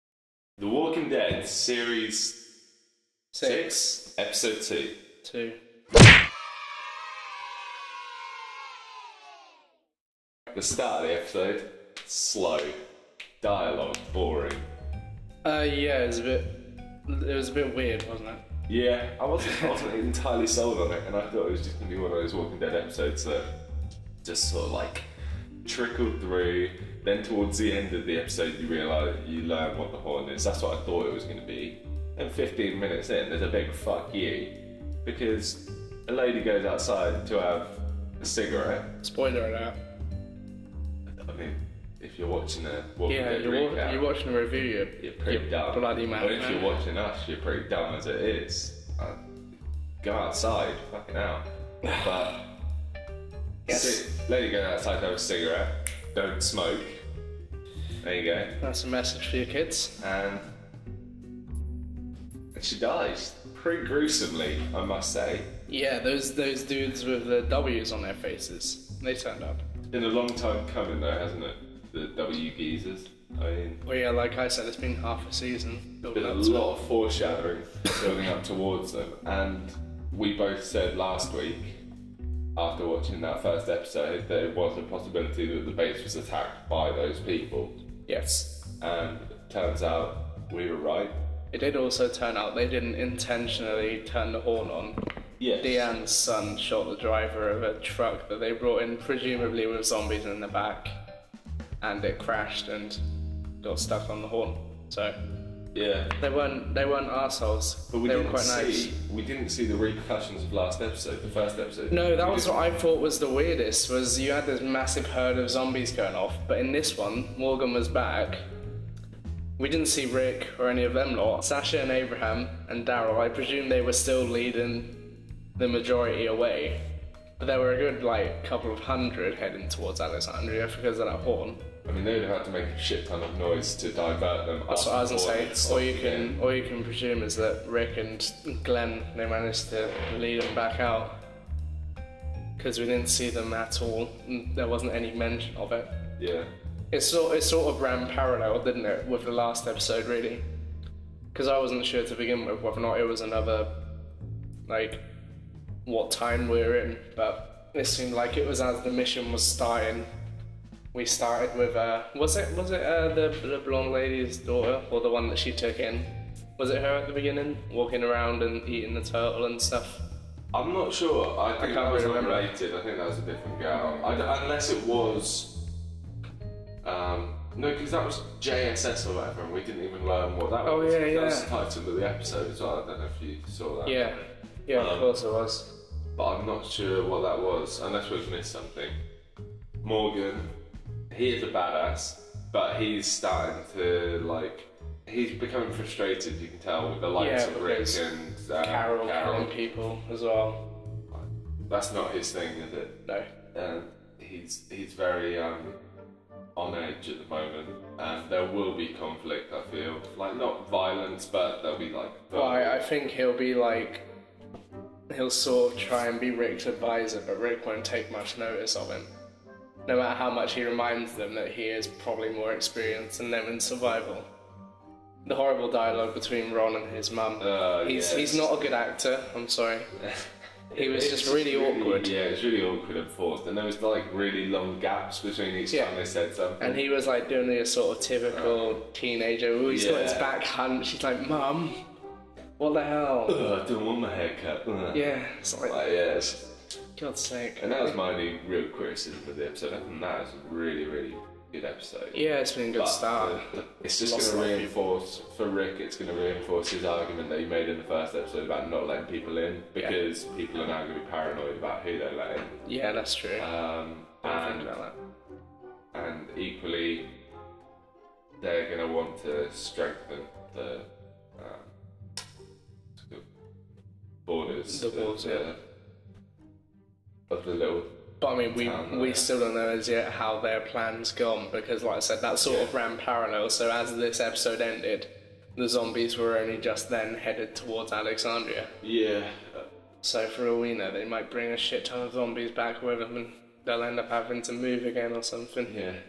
The Walking Dead season 6 ep 2 - review
Today we ramble on about episode 2 of the Walking Dead, putting across our uneducated opinions for YouTube to see!